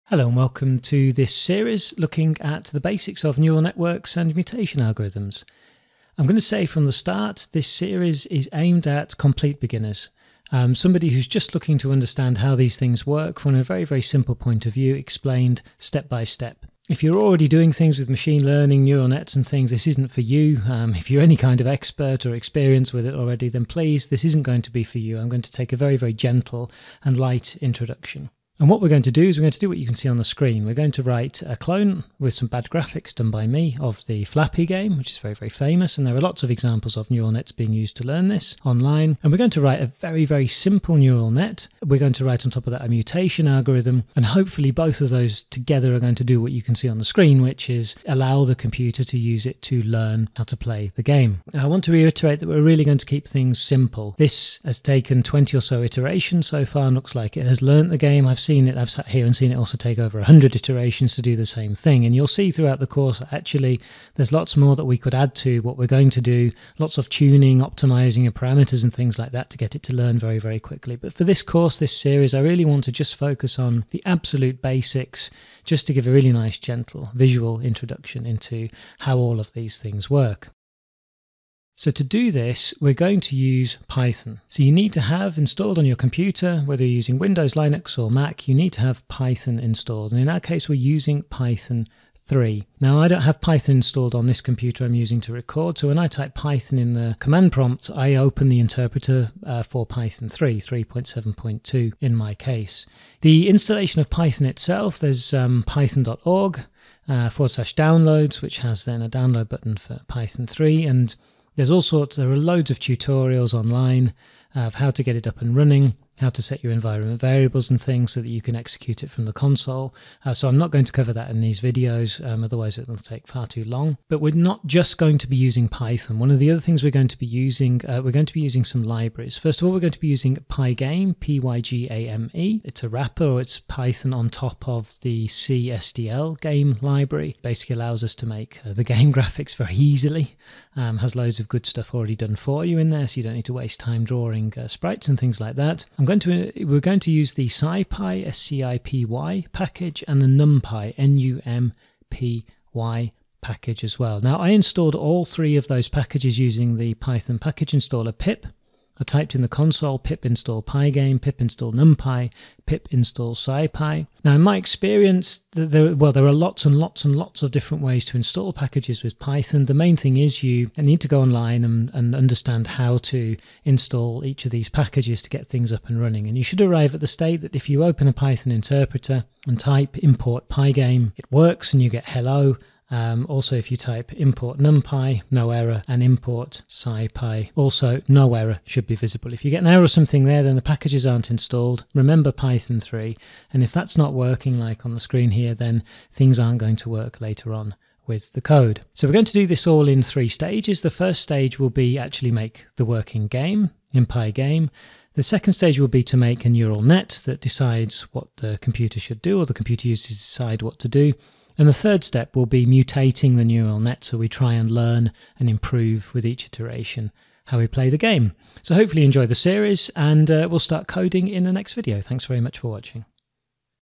Gentle Intro To Neural Nets #1 Learning To Play A Game
A gentle intro to Neural Nets - Flappy and pygame.

Short series to help you get started with neural networks and mutation algorithms by teaching the computer to play Flappy